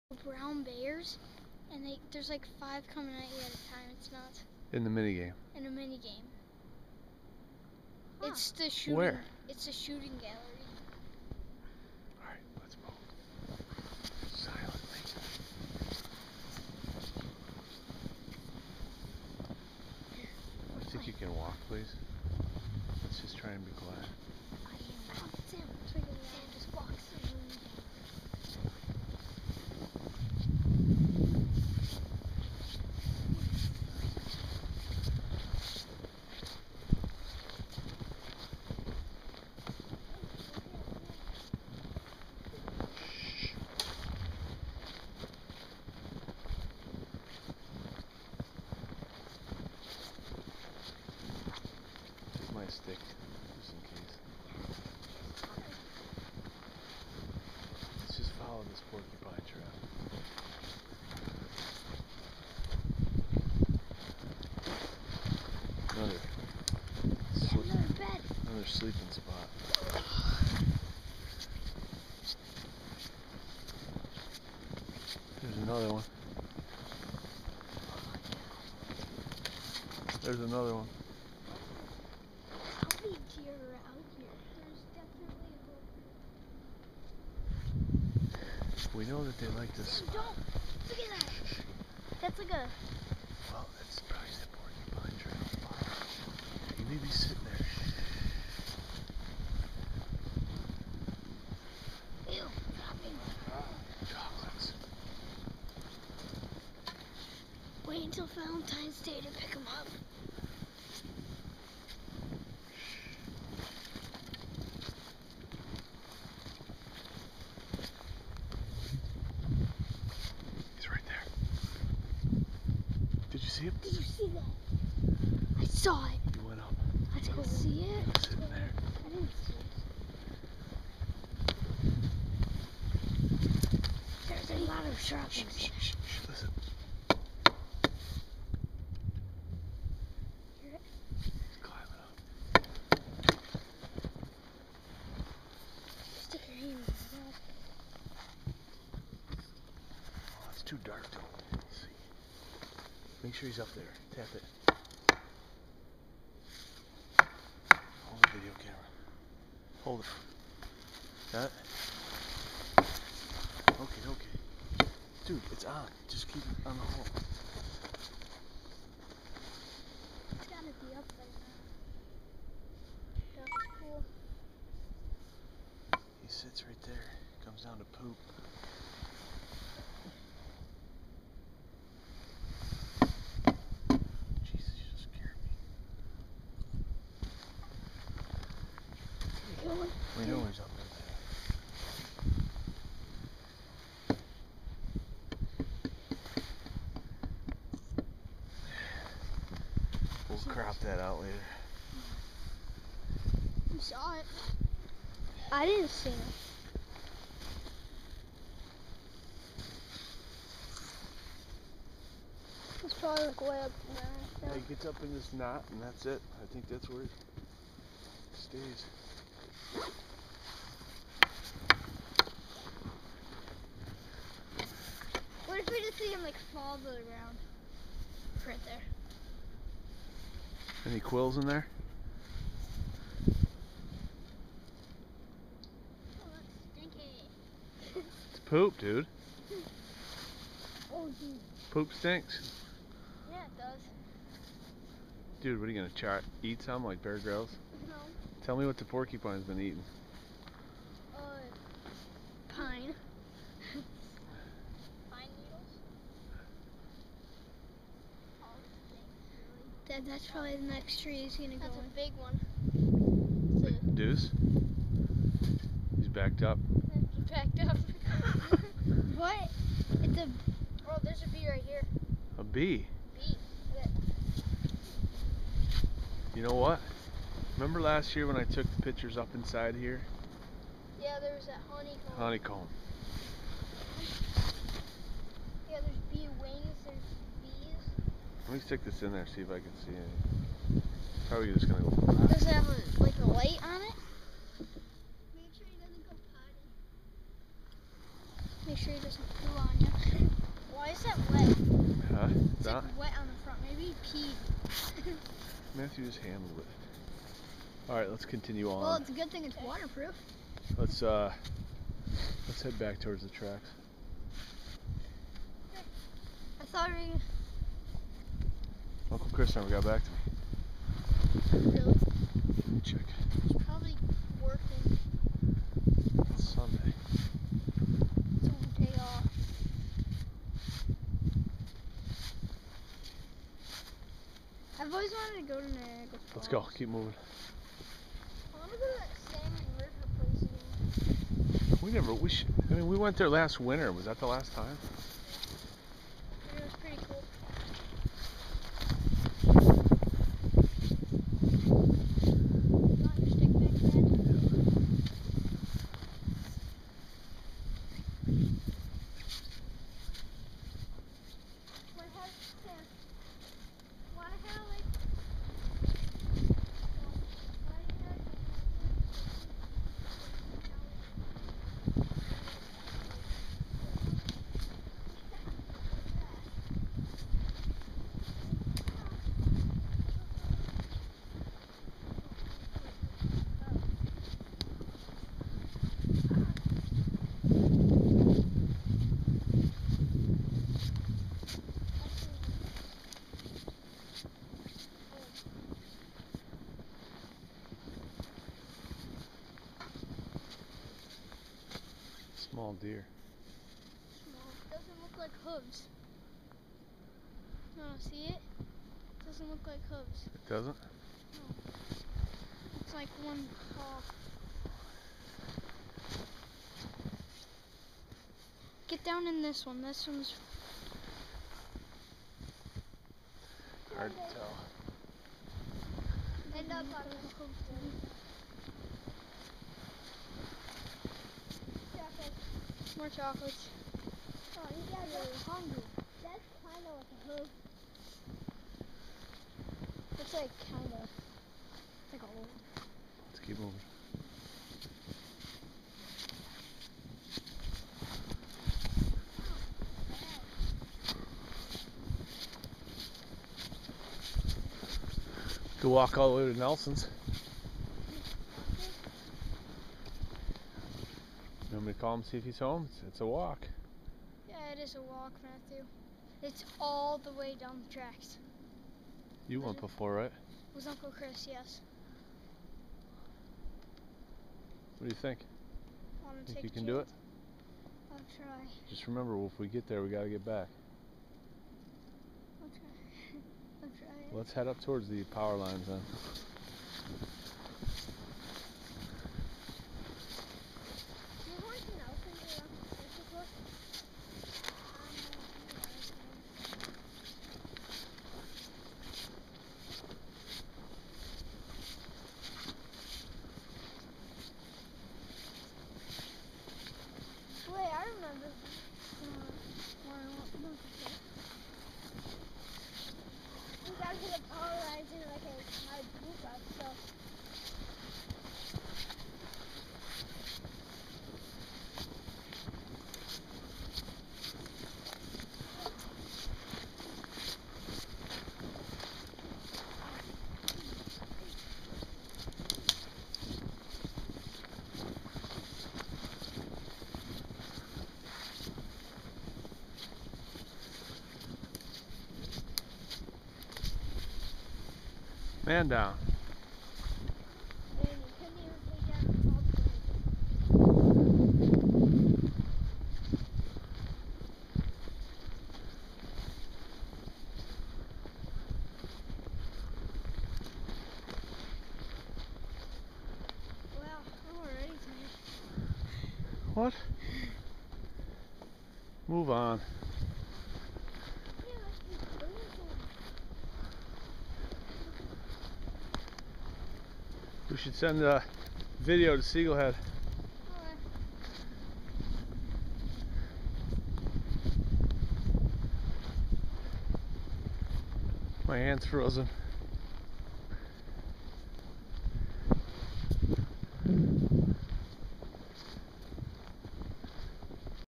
Winter walk in the woods
What a nice day for a walk in woods with the boys.  Good stuff!